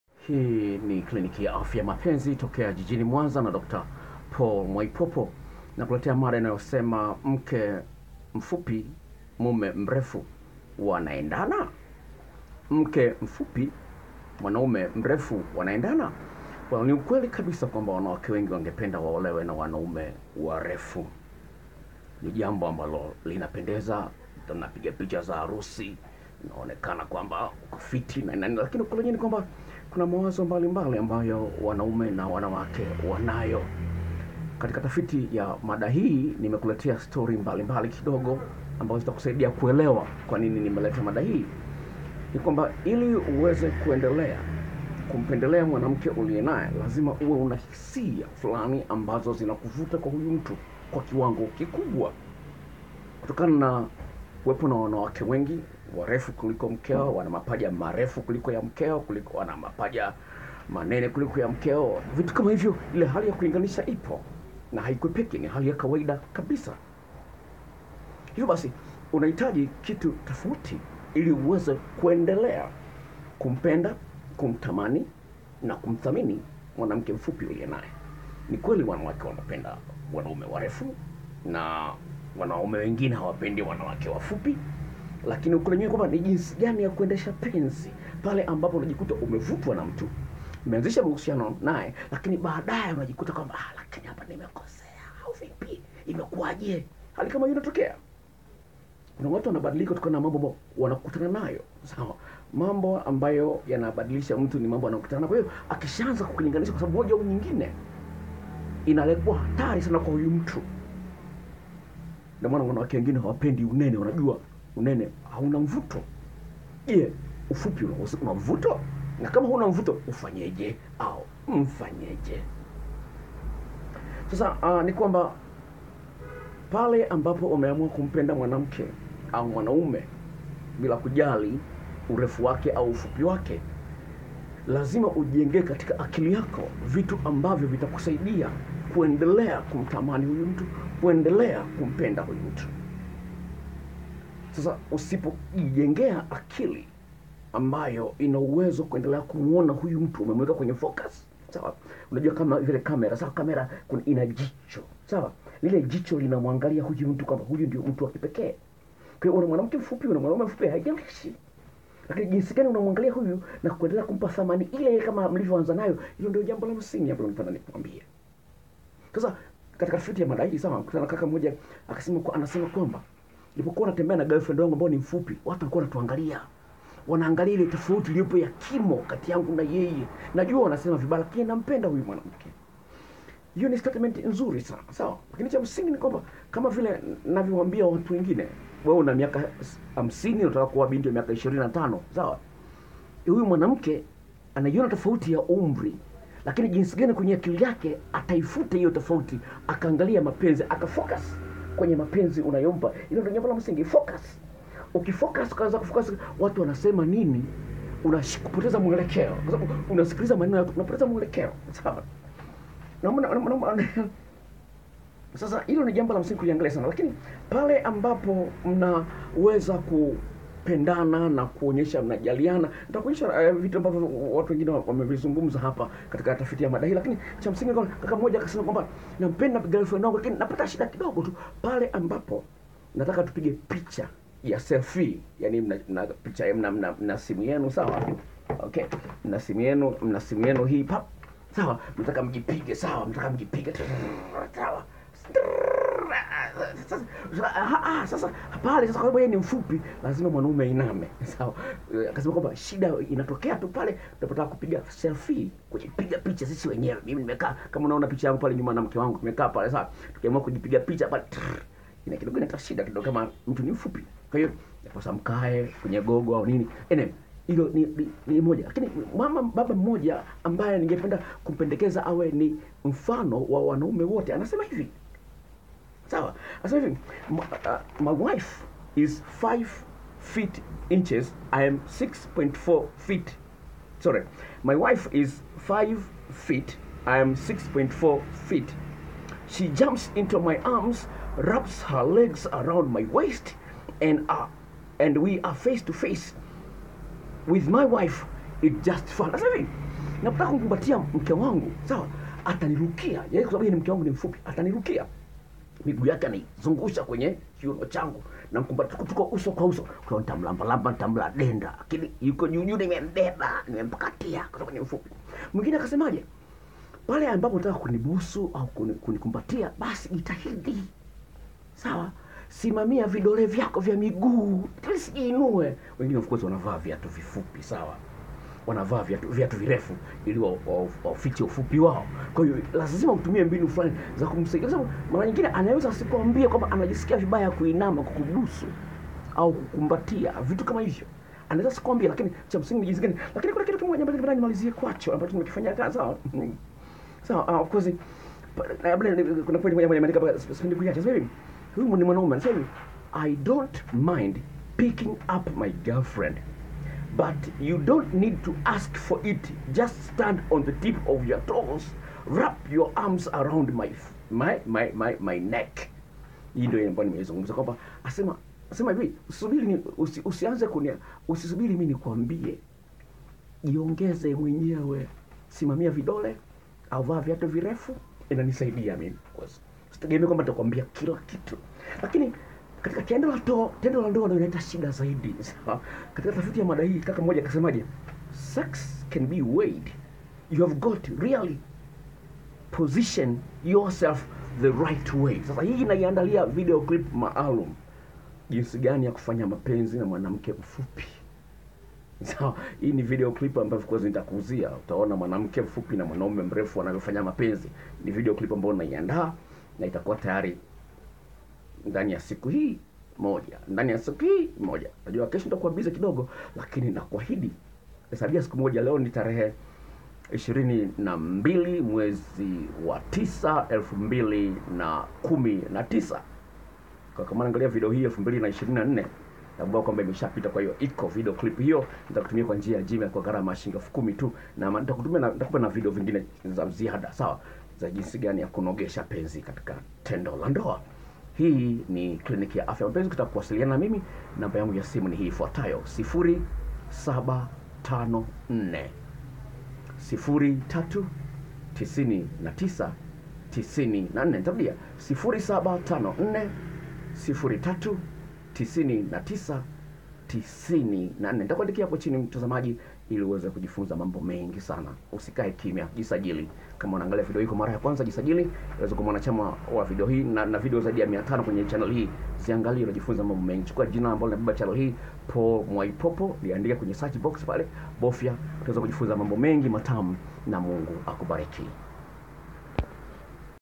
Mke mfupi mume mrefu wanaendanaje? DR Paul Mwaipopo
Kimo kimewaletea shida watu wengi sana na leo nakupa njia za kuepukana na majanga ya tofauti ya kimo TUWASILIANE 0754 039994